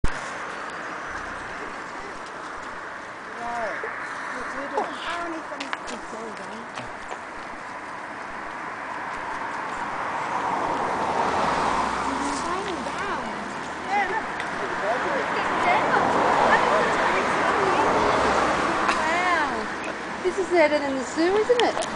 Koala mommy and baby
Koala with baby in the wild at Hawks Nest, Port Stephens, NSW, Australia.  October 2007.